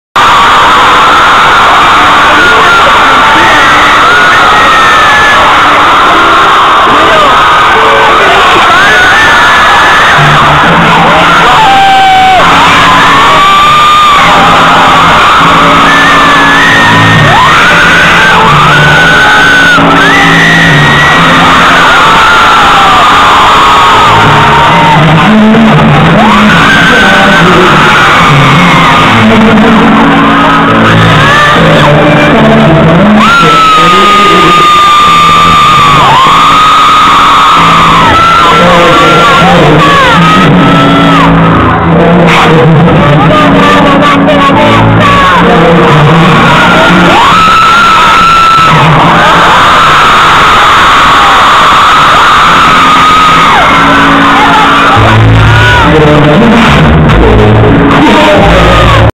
Concierto de Junsu. part 13
Concierto de Junsu en Santiago de Chile. Sorry por los gritos.